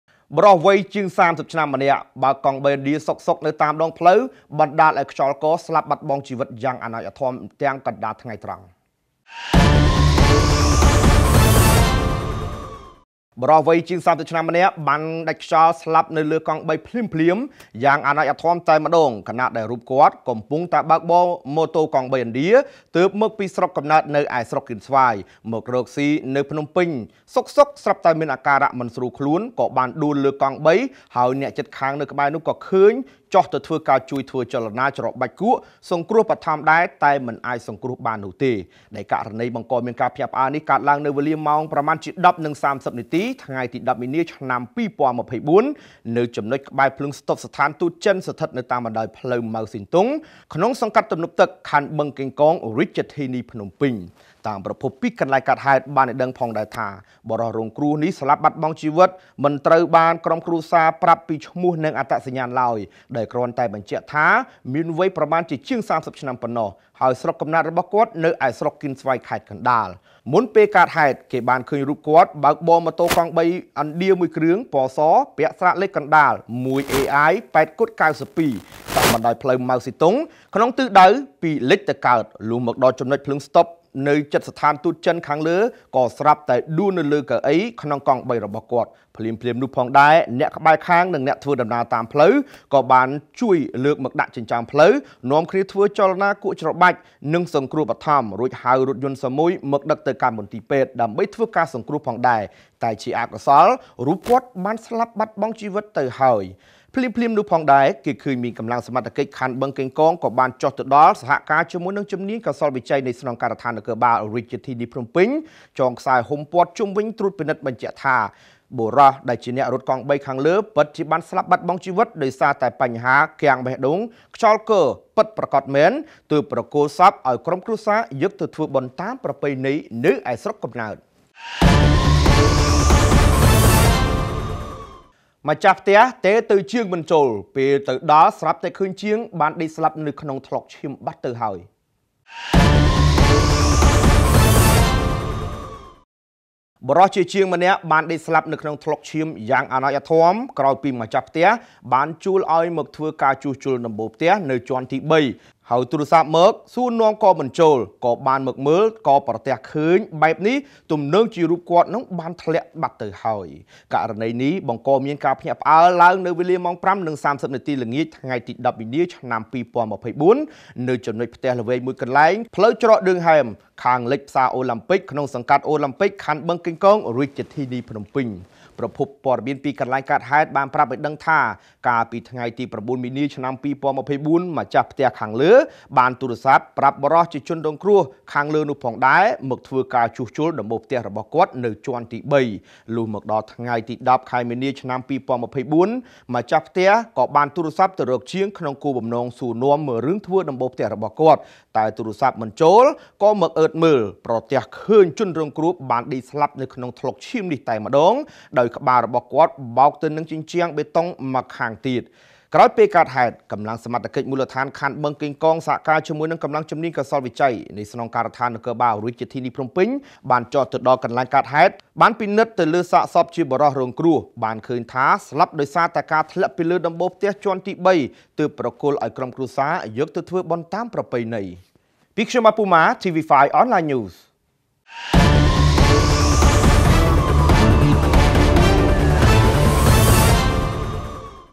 បុរសវ័យជាង ៣០ ឆ្នាំម្នាក់បើកកង់បីឥណ្ឌាសុខៗខ្យល់គ ស្លា..ប់បាត់បង់ជីវិតកណ្តាលផ្លូវយ៉ាងអាណោចអាធ័ម
- បុរសវ័យជាង ៣០ ឆ្នាំម្នាក់ បើកកង់បីឥណ្ឌាពីស្រុកកៀនស្វាយ មករត់នៅភ្នំពេញ បើកសុខៗខ្យល់គ ស្លា..ប់បាត់បង់ជីវិតកណ្តាលផ្លូវយ៉ាងអាណោចអាធ័ម
- ម្ចាស់ផ្ទះទូរស័ព្ទមិនចូល លុះមកមើលឃើញ បុរសជាជាងដេកស្លា..ប់ក្នុងថ្លុកឈា..ម
ខណៈដែលបានជួលឱ្យមកជួសជុលដំបូលមួយកន្លែងនៅជាន់ទី៣ ក្បែរផ្សារអូឡាំពិក